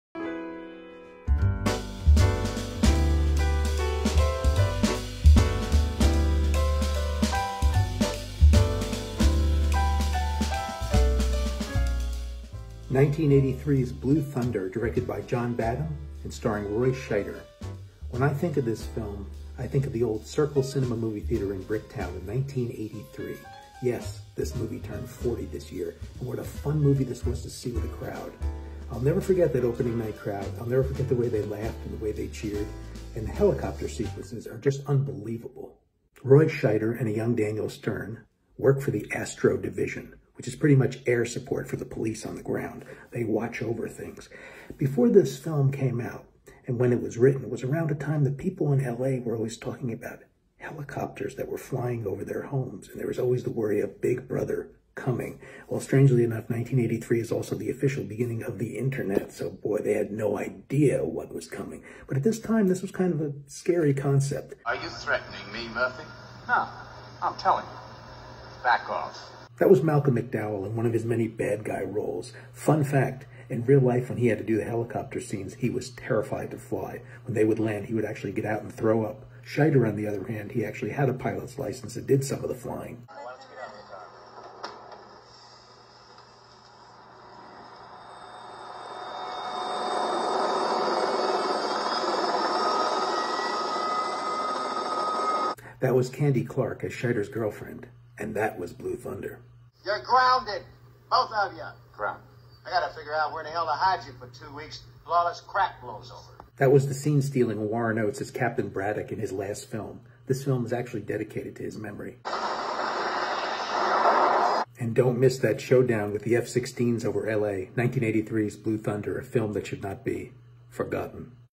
Forgotten Film 1983's Blue Thunder
Episode 18 of my Forgotten Film series is 1983’s Blue Thunder starring Roy Scheider!